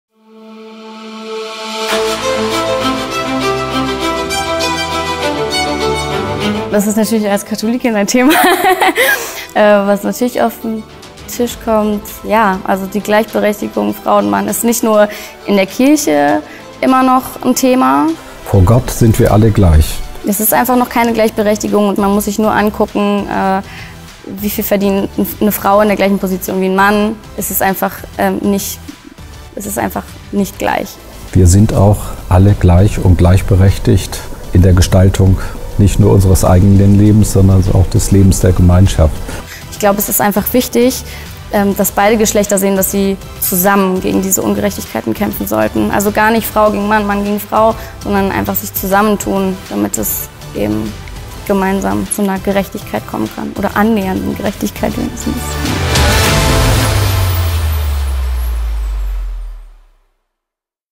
Erbe der Reformation: Gleichberechtigung
"Die Gleichberechtigung von Frau und Mann ist nicht nur in der Kirche immer noch ein Thema", meinen Bremens Reformationsbotschafter Jens Böhrnsen und Maria Esfandiari. Doch vor Gott sind wir alle gleichberechtigt und frei in der Gestaltung unseres Lebens und unserer Gemeinschaft. Ein Appell, nicht gegeneinenander, sondern miteinander für Gleichberechtigung zu kämpfen.
Frei denken - Frei glauben - Frei leben, für Gleichberechtigung und ein Miteinander auf Augenhöhe.

Produktion: Evangelischer Kirchenfunk Niedersachsen GmbH (ekn)
Redaktion: Sabine Hatscher